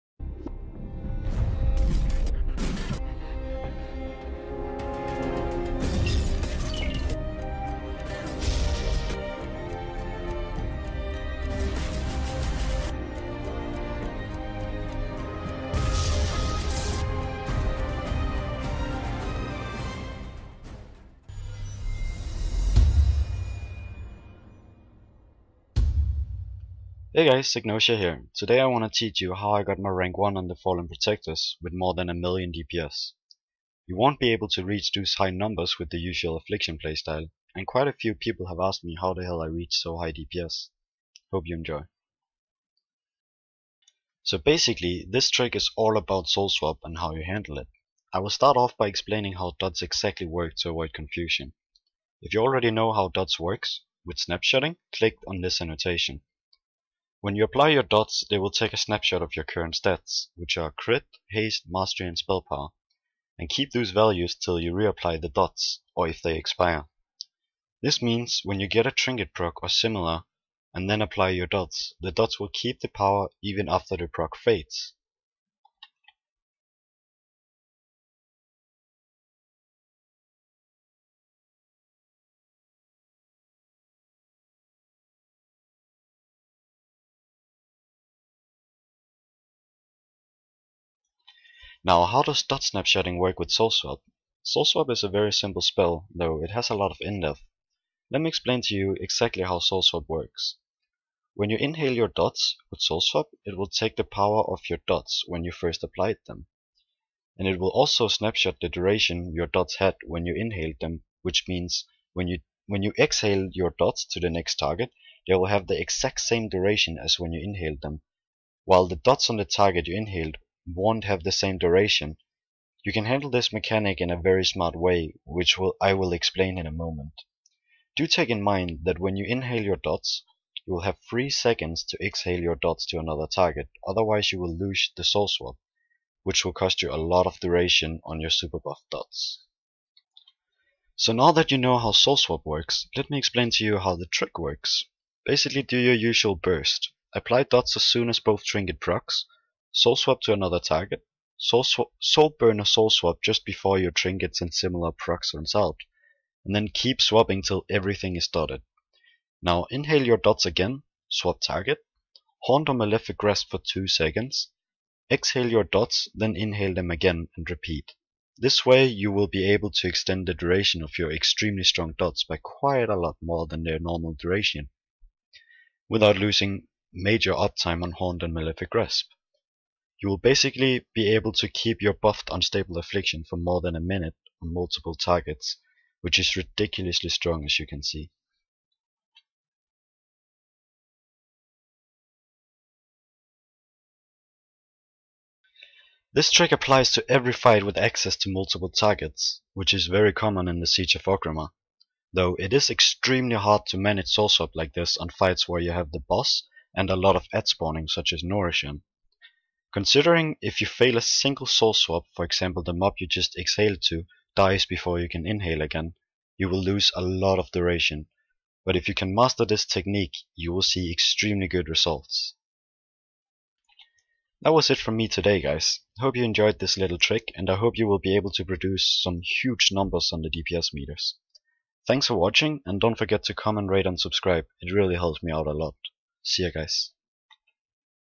How to MASSIVELY increase your DPS as Affliction (1 million+)
Please give me feedback regarding how well this works for you guys, would appreciate it :)